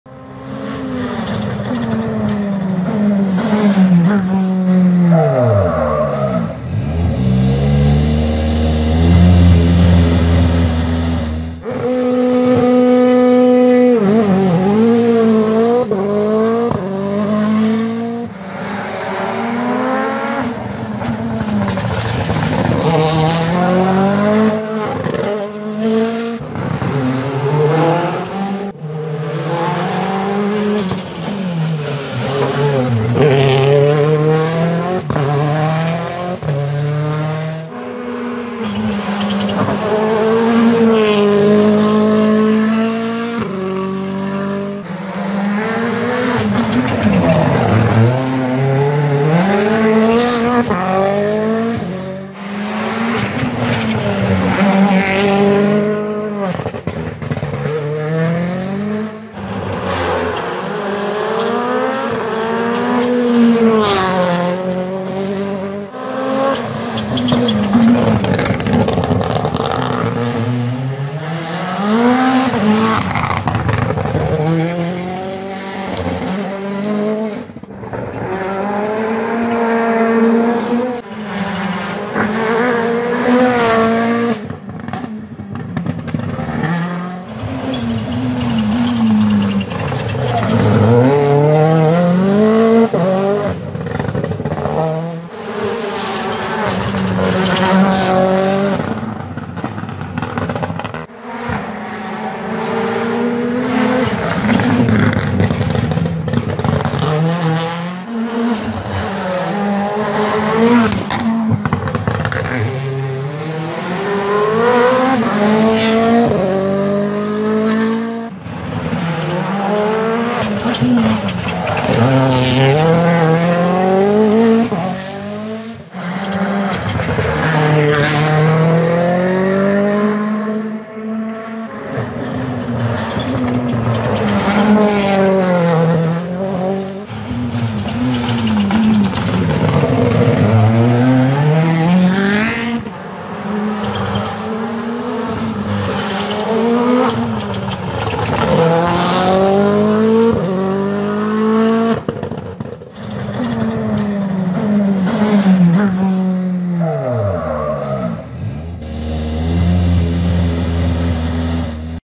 Test Hyundai i20 N Rally1 E Lappi Rallye Monte Carlo
Test du 11 janvier en vue du Rallye Monte Carlo 2023 de Esapekka Lappi sur Huyndai i20 N Rally1 Hybrid sur route sèche.